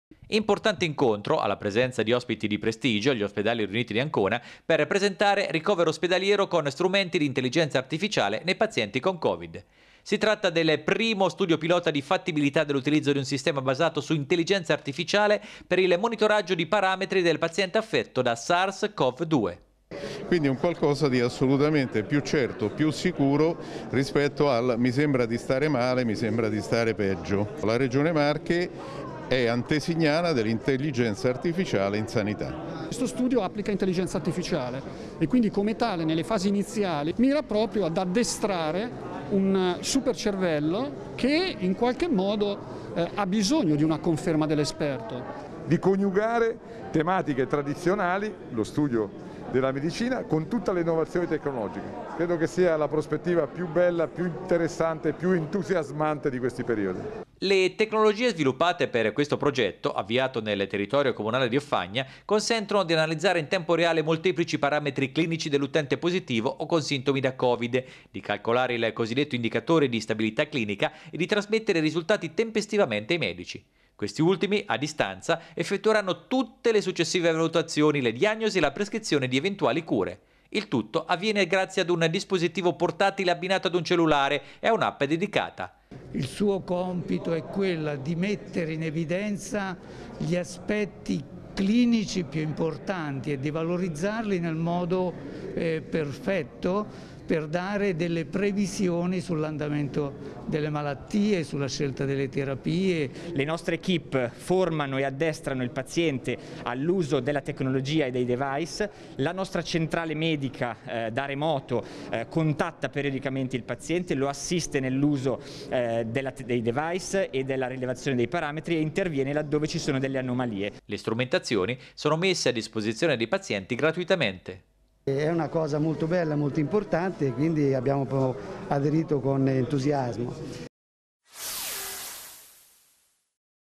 Ancona - Intelligenza artificiale per monitorare i positivi al covid, presentato lo studio pilota
I medici potranno prescrivere le cure anche da casa